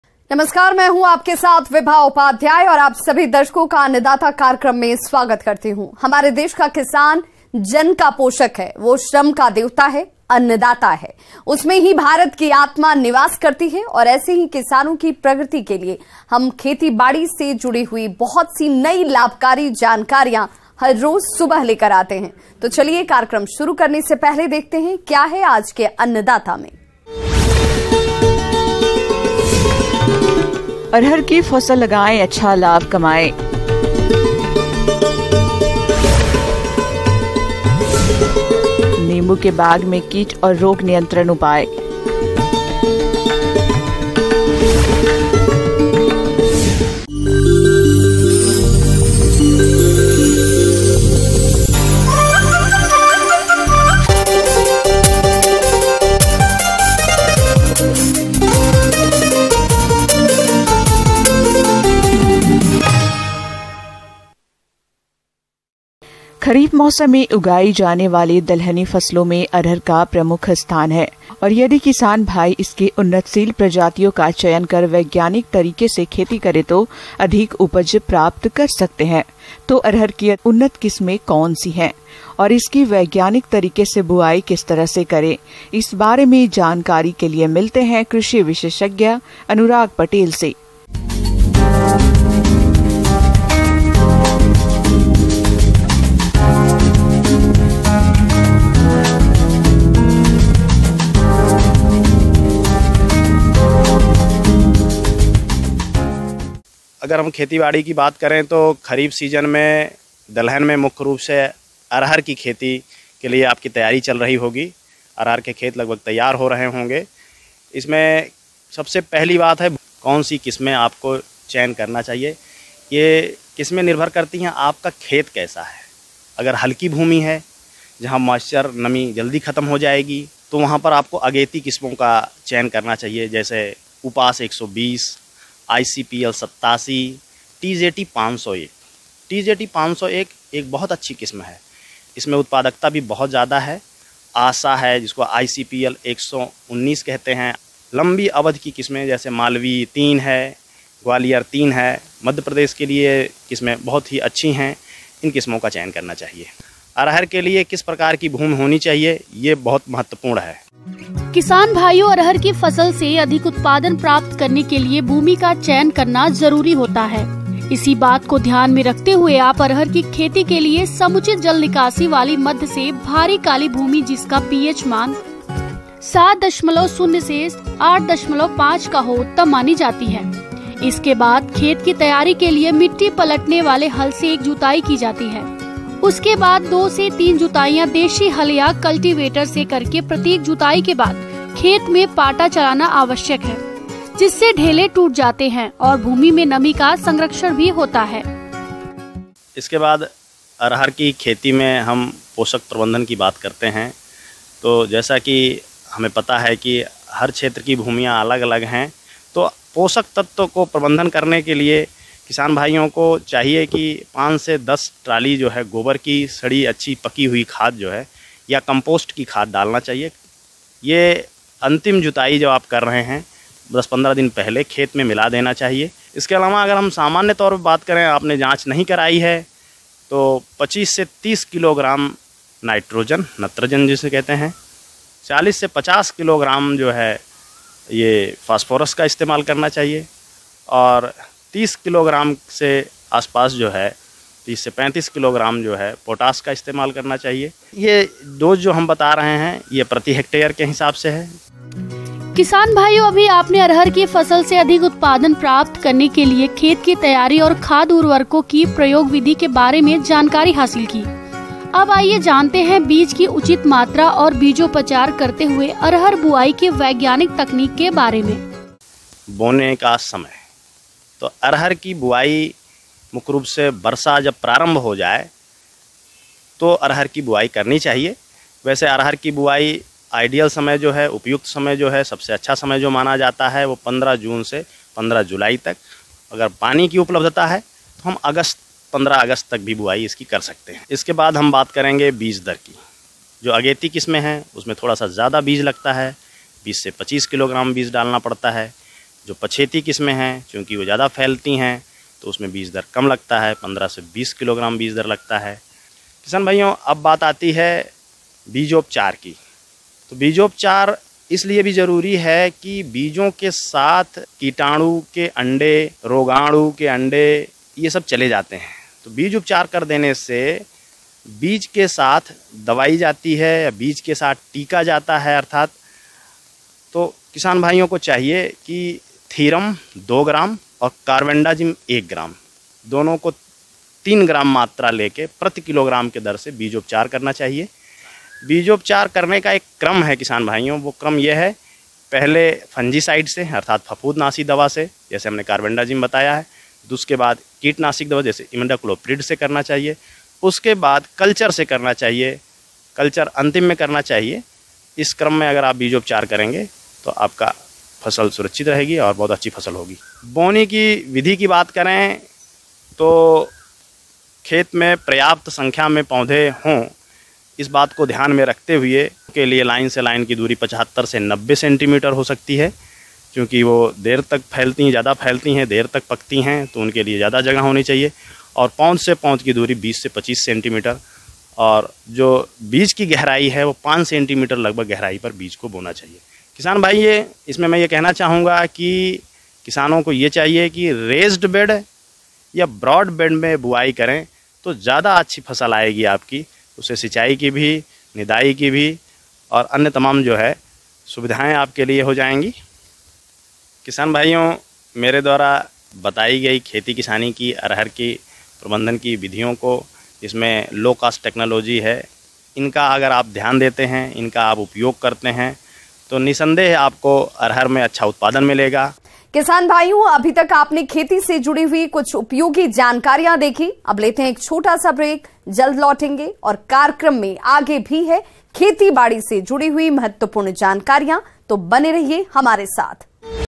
Annadata | अधिक उपज देने वाली अरहर की उन्नत किस्मों का करें चयन, कमाएं अच्छा लाभ | Top News | News18
Annadata | खरीफ मौसम में उगाई जाने वाली दलहनी फसलों में अरहर का प्रमुख स्थान है. ज्यादा जानकारी के लिए देखिए